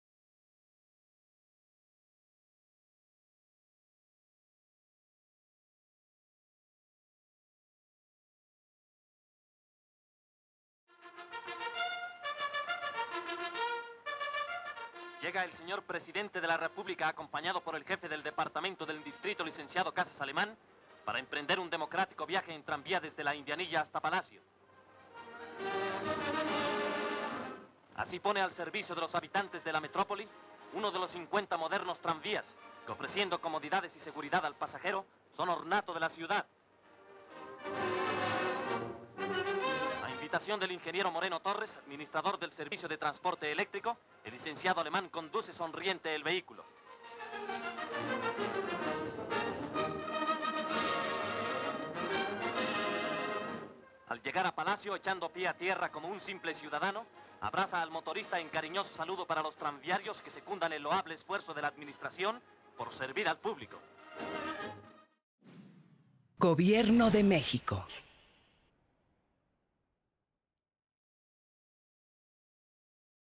De Indianilla a Palacio. Año 1947. Noticiero mexicano, N778. Película de nitrato, 35 mm.
Con motivo de la entrega de cincuenta tranvías nuevos para la Ciudad de México, el presidente Miguel Alemán y Fernando Casas Alemán, jefe del Departamento del Distrito Federal, realizan un viaje desde la estación Indianilla (actualmente Colonia Doctores) al Palacio Nacional. En un tramo el presidente toma el volante para cersiorarse de su buen funcionamiento.